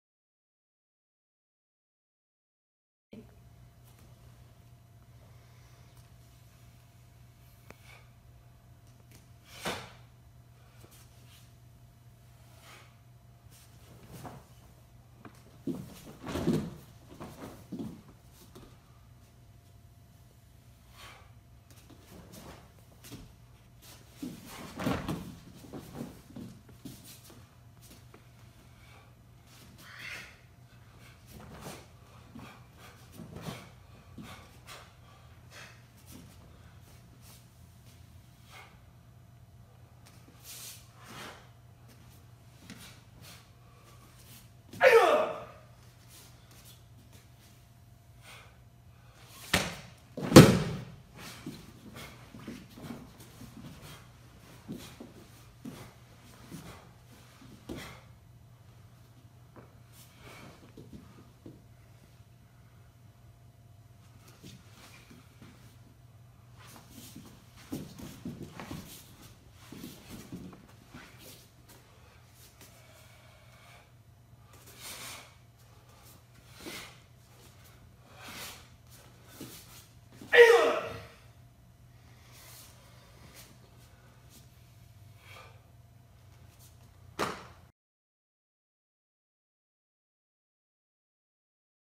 Wizdom - Master Jeff Wade
Master Jeff Wade performs Wizdom, SMTKD 5th dan form